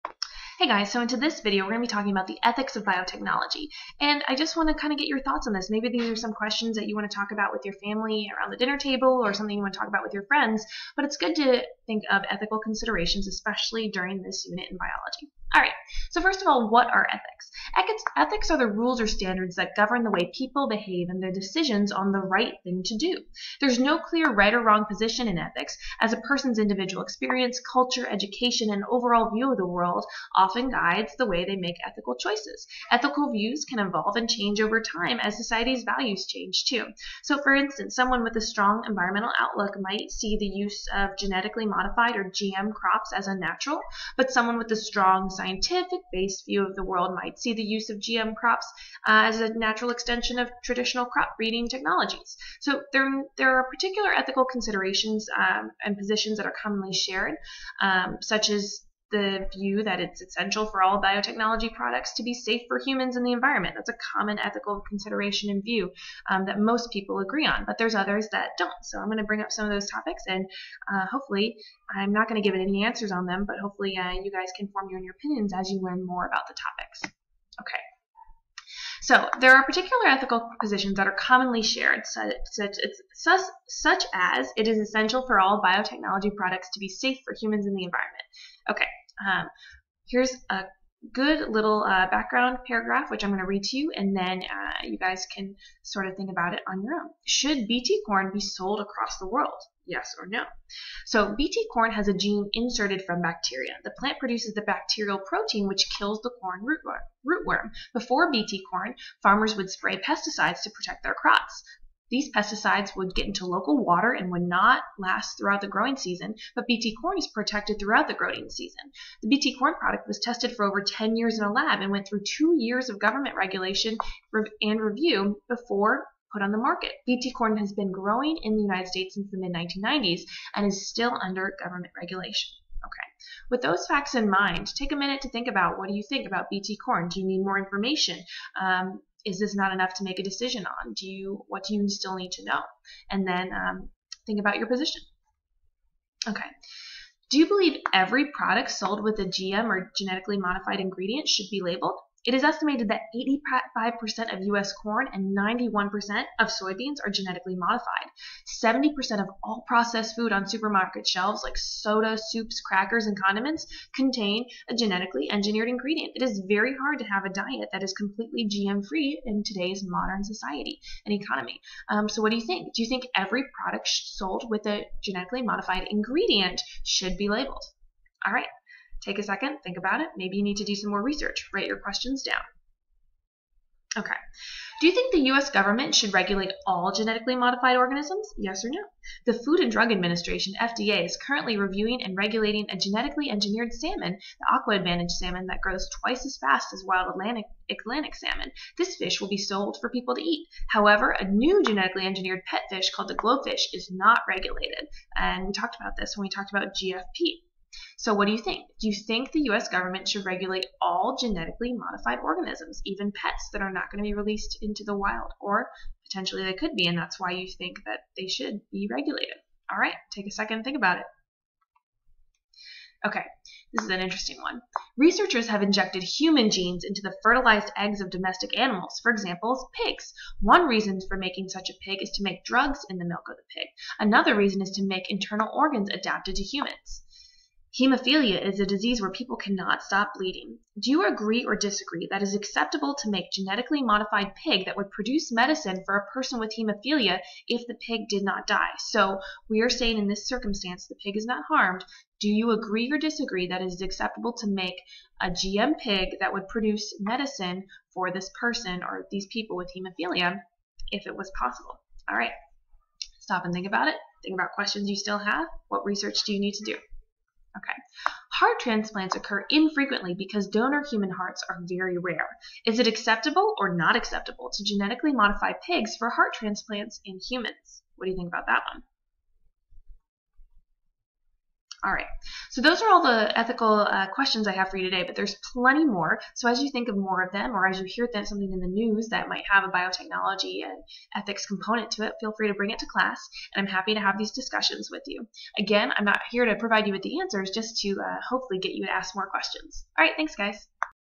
Ethics of Biotechnology - Questions to Consider: Part 1
This video is Part 1 of 2 in my Bioethics series! Be sure to check out Part 2, where I've updated my bioethical questions for 2021:

Ethics are the rules or standards that govern the way people behave and their decisions on the ‘right thing’ to do.

There is no clear right or wrong position in ethics, as a person’s individual experience, culture, education, and overall view of the world often guides the way they make ethical choices.

Ethical views can evolve and change over time as society’s values change.

In this video, I'll go over an introduction to bioethics, pose some popular questions and cases for ethical choices in biotechnology, and provide you an opportunity to think about these issues yourselves!

00:00​ What are ethics?
00:55​ Common shared ethical beliefs
01:29​ Ethical question: Genetically modified plants/Bt corn
02:26​ Ethical question: GMO Labeling in Food
0:03​ Ethical question: US Regulation of Glofish
03:43​ Ethical question: Pigs & Medicine
04:27​ Ethical question: Pigs & Human Organs